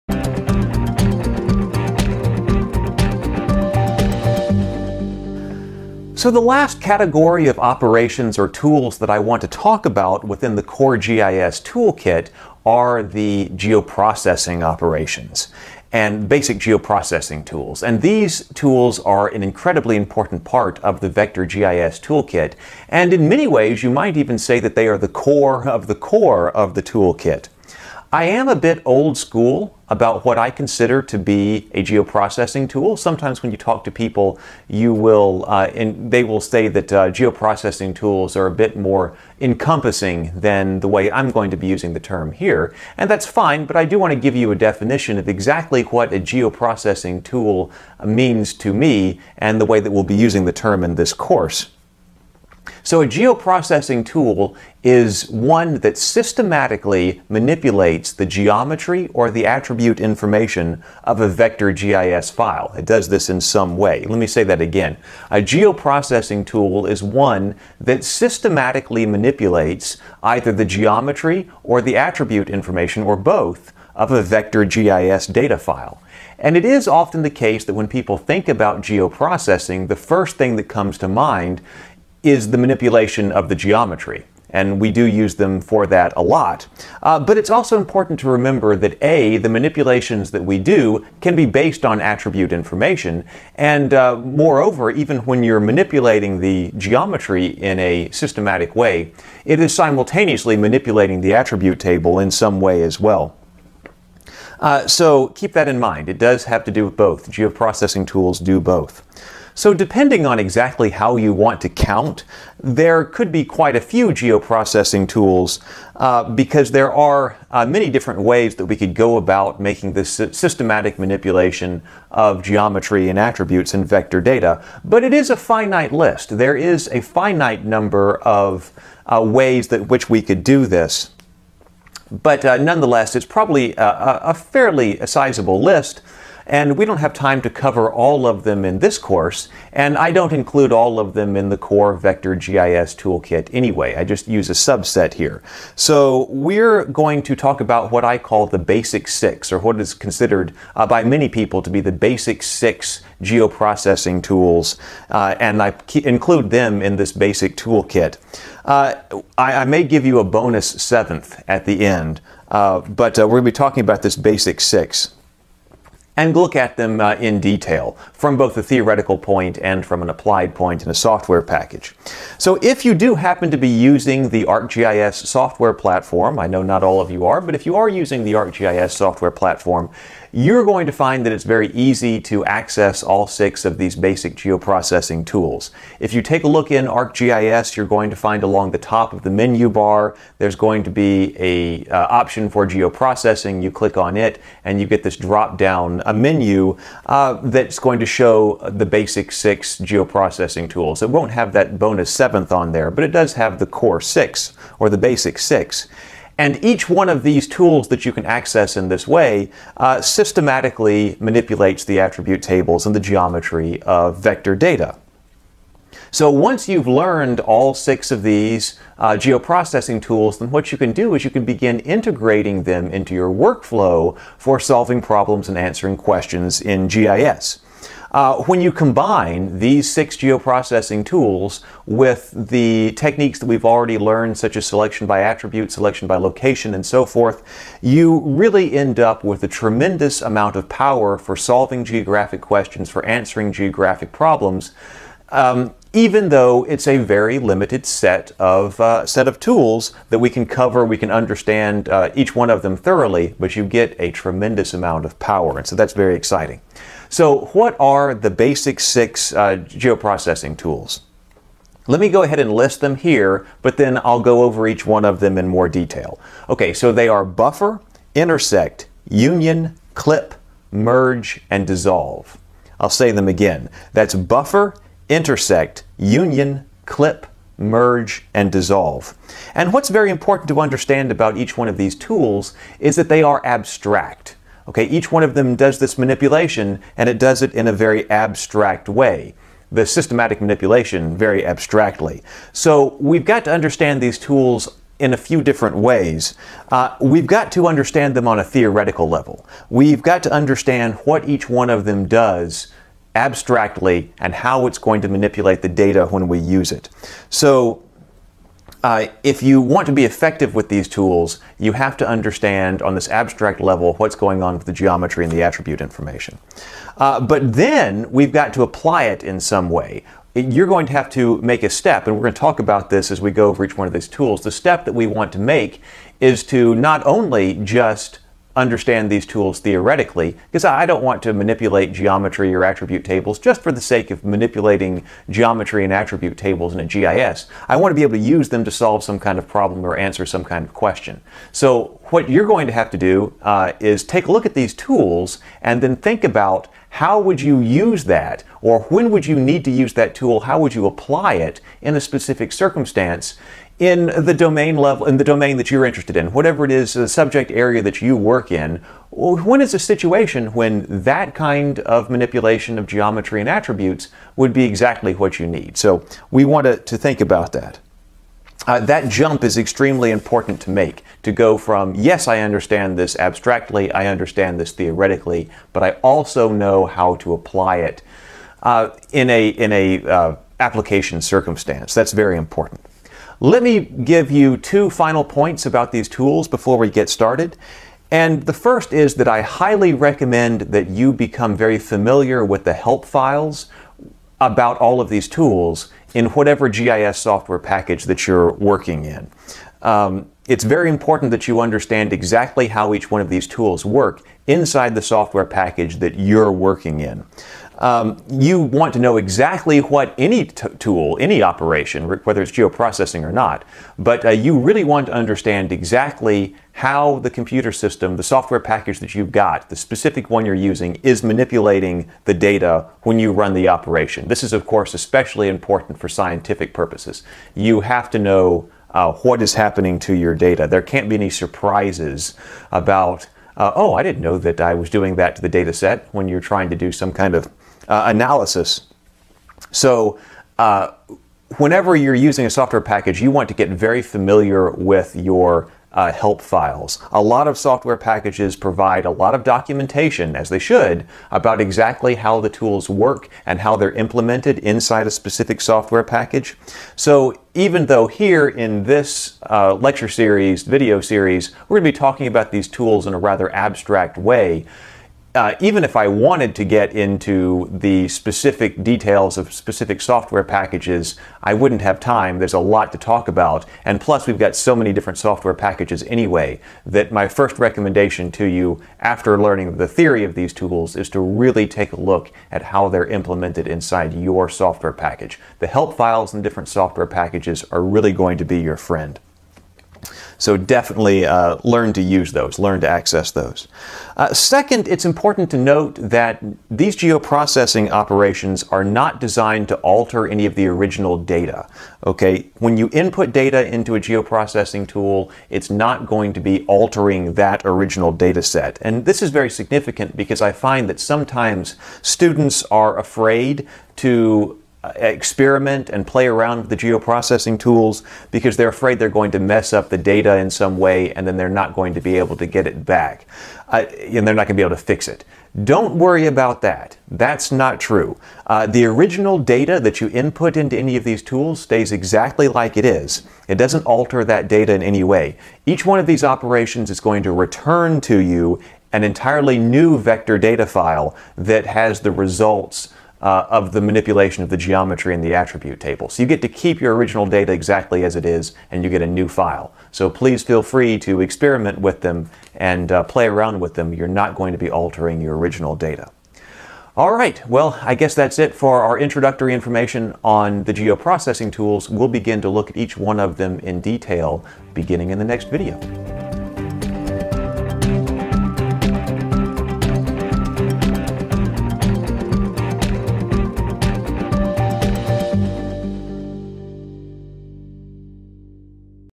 Intro to GIS Geoprocessing - The Core Vector GIS Toolkit (7)
Geoprocessing is one of the most fundamental ways that we conduct vector GIS analysis. The geoprocessing tools are a suite of tools that, while individually abstract, are extremely powerful when understood and applied. It is critical that GIS analysists understand each of these tools, what input they accept, and what output they return.